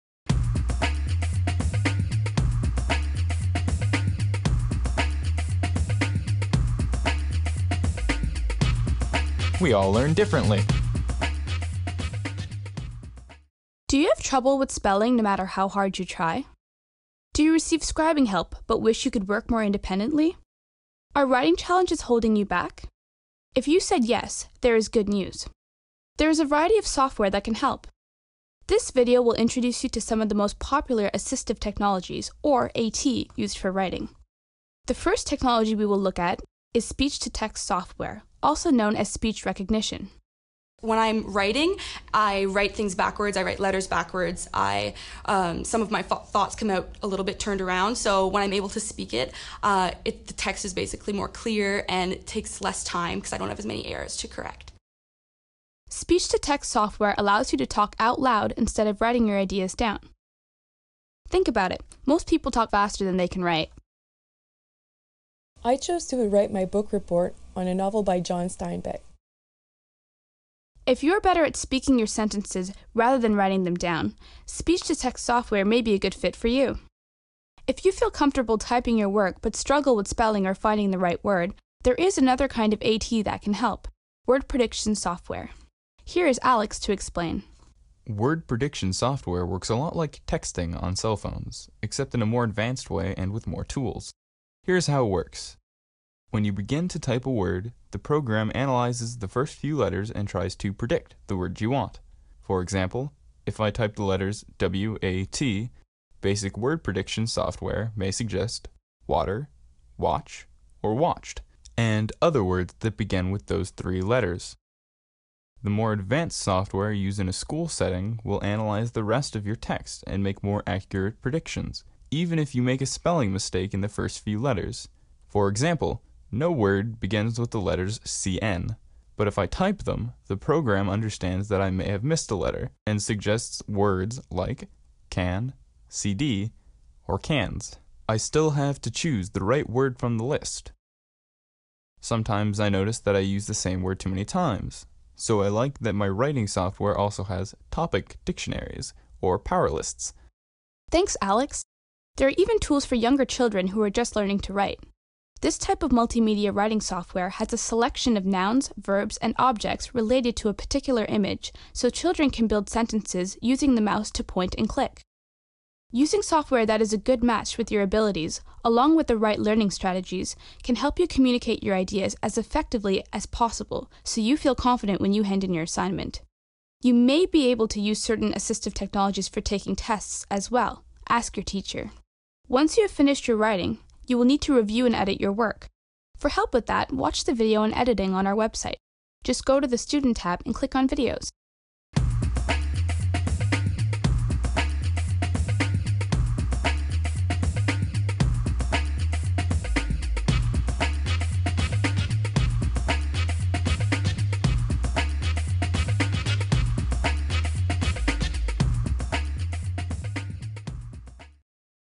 K-12 AT Tools for Writing
Assistive technology software programs in combination with  learning strategies are great writing tools for students.